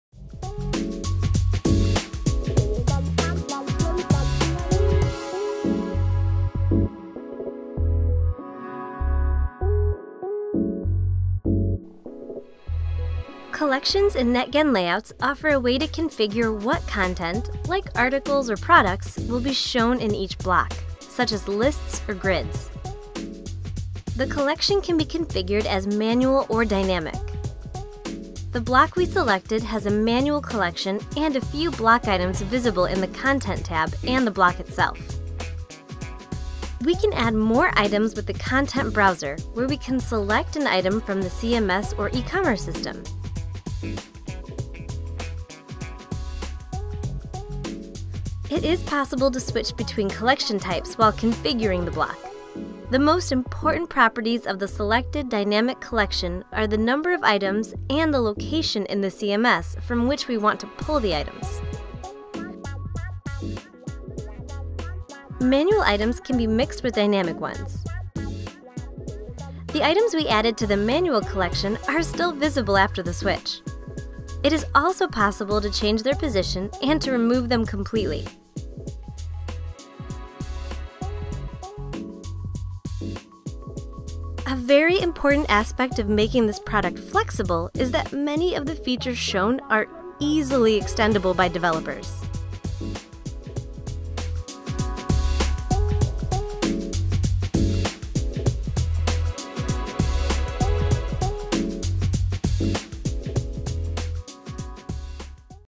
Netgen Layouts tutorial 1 – Working with collections and content
Netgen Layouts is a powerful website building tool based on Symfony PHP framework for managing all page layouts on your website.

In this tutorial video, learn how to work with collections and content.